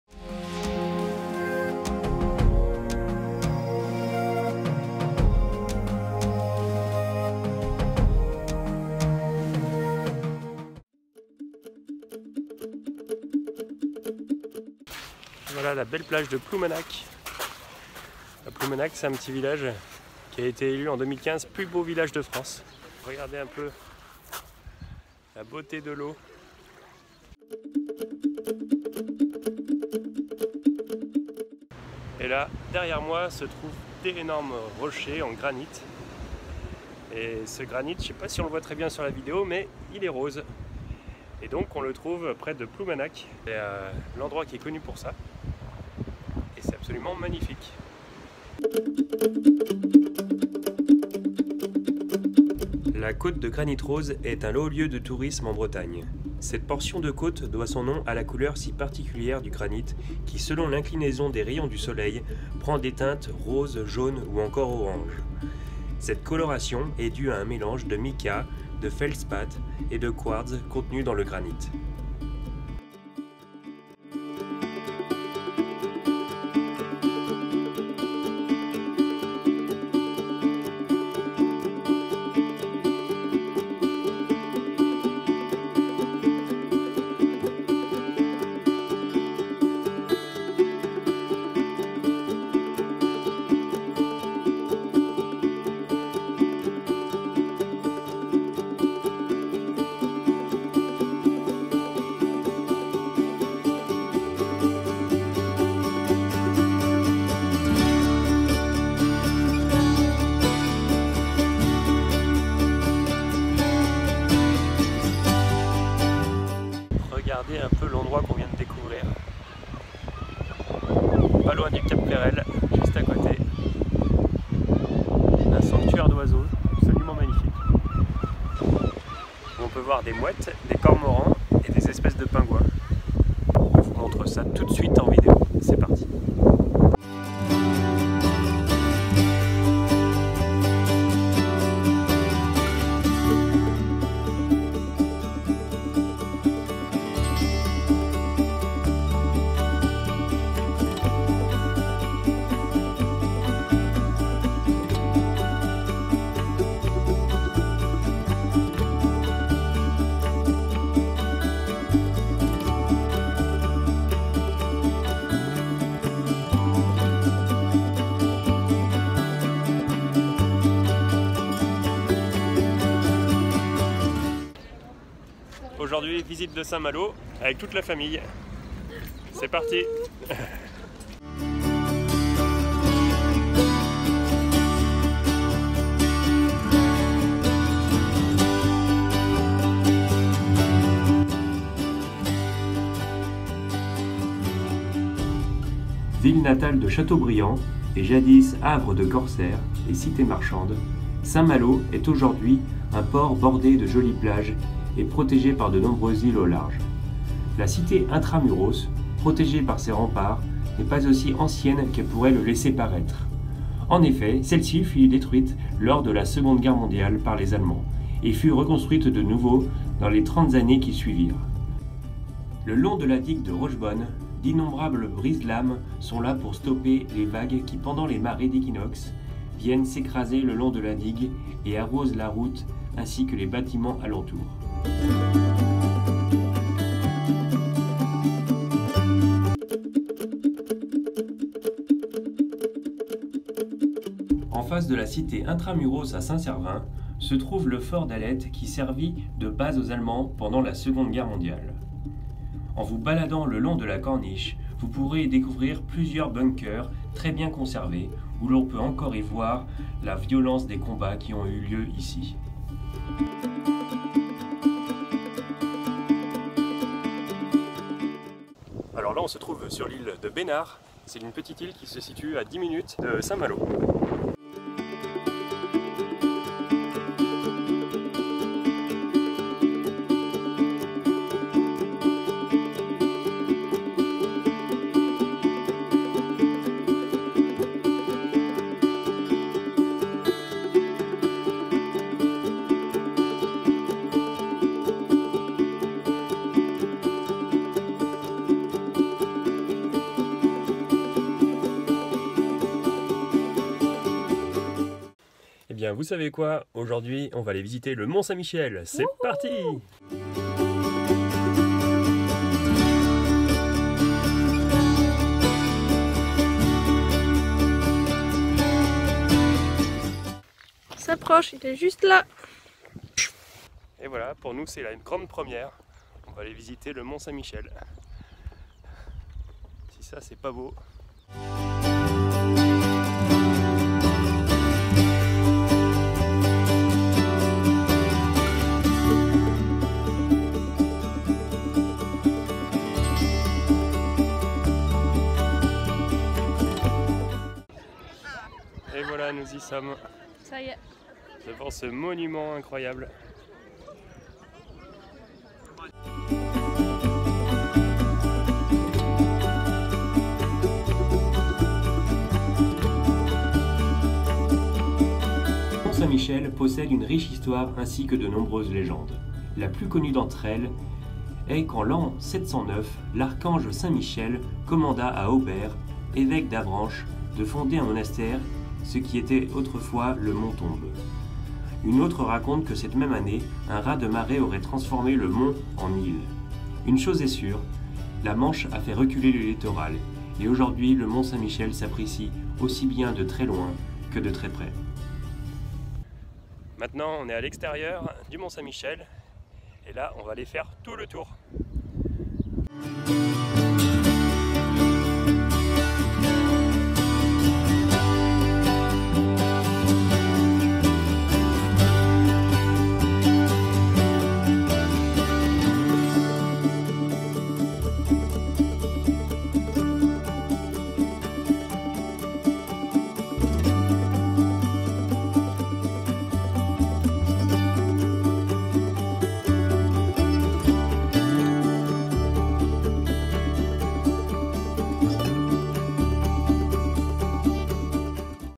VISITE D'UNE CITÉ CORSAIRE 🏴 ☠ - FRANCE - Nomades 2.0
Première étape le village de Ploumanac'h et sa côte de Granite Rose, suivie par la découverte du Cap Fréhel et son sanctuaire d'oiseaux marins !
Nous enchainons avec la visite de la ville de Saint Malo avec son Intra Muros et sa digue, ainsi que la cité d'Aleth et de l'île Besnard. Nous terminons par l'un des plus beaux monument de France l'incroyable Mont Saint Michel !

Vlog voyage 46 I France I Tour d'Europe

Départ depuis le 29 avril 2018 des Sables d'Olonne en Vendée, pour un tour d'Europe en van !
La vidéo se déroule du 21 au 27 mai 2018

LIEUX VISITÉS :

Ploumanac'h
La Côte de Granite Rose
Le Cap Fréhel
Saint Malo
La cité d'Aleth
L'Île Besnard
Le Mont Saint Michel

REMERCIEMENTS :

Christelle et Yves à Guildo pour nous avoir hébergé une nuit et nous avoir offert une soirée mémorable arrosé au whisky !

Fanny, Jean-Wilfrid, Paul et François les Maloins (Saint Malo) pour nous avoir hébergé durant 3 jours et nous avoir fait visiter la région, encore un grand merci à vous 4.

Chantal et Jean-Patrick pour nous avoir invité à l'apéro à Touraude prêt de la Rance. Encore un très bon moment !